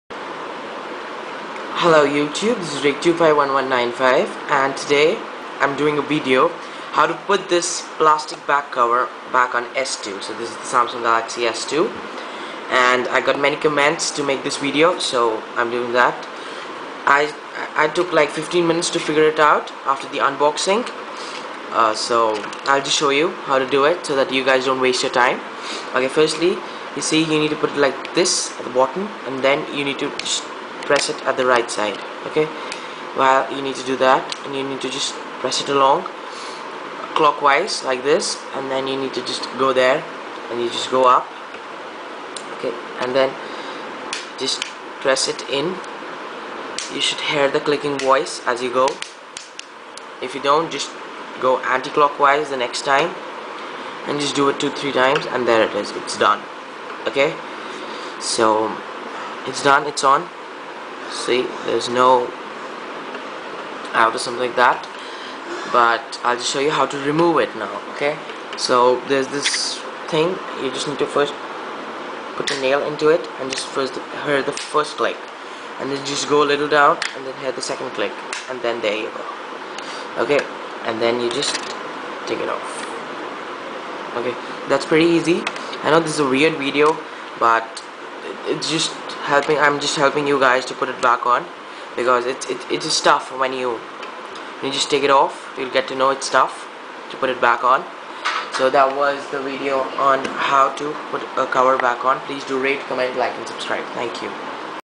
How to put the Back cover on Samsung Galaxy S2
It took me a while to figure this out .. :P The proper Way..